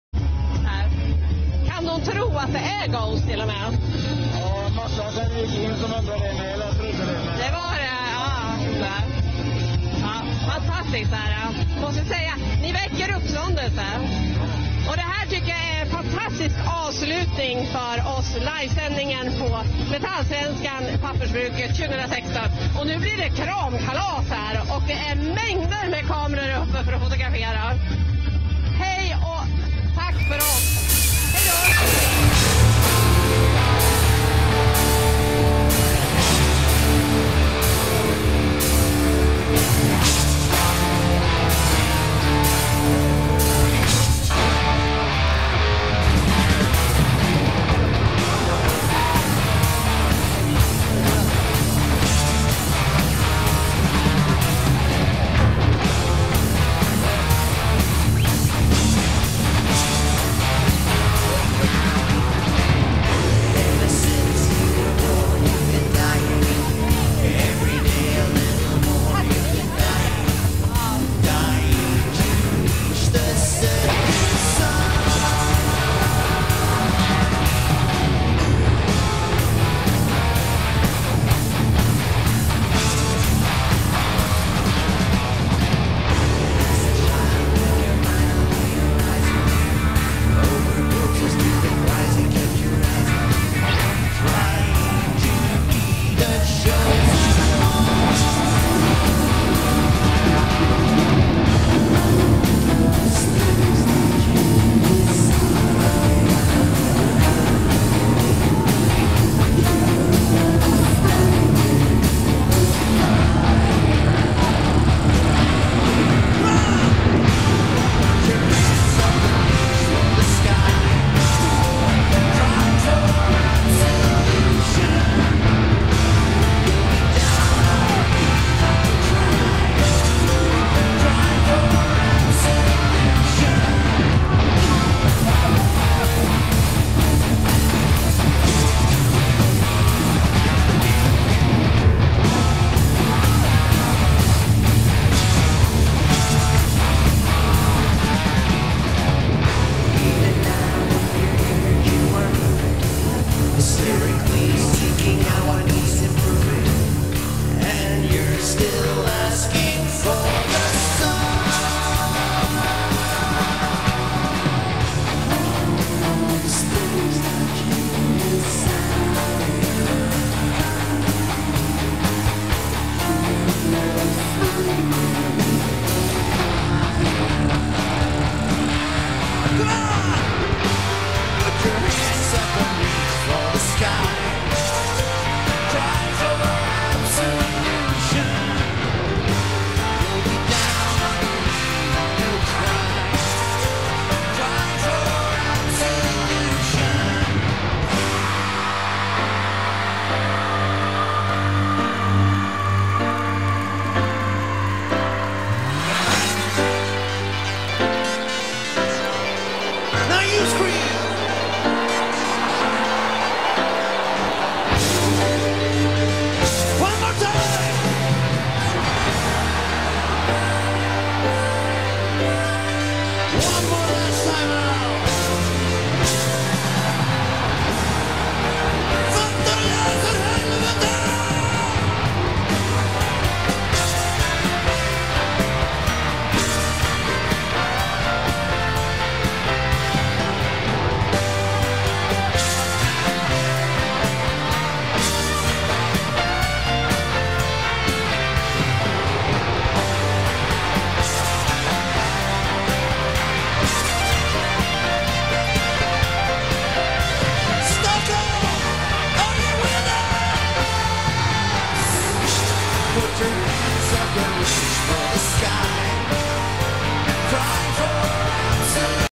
Ghost - Dressed Up Fans Interviewed @ Metallsvenskan 2016
Me and a few friends of mine in Sweden decided to go in our costumes to see Ghost play at Metallsvenskan, NA heard rumours about our costumes and wanted to give us an interview and ofcourse we said yes and during the interview we were stormed with people who mostly thought we were the band so we had a lot of pictures taken and hugged and talked to a lot of awesome people !
For more of my stuff!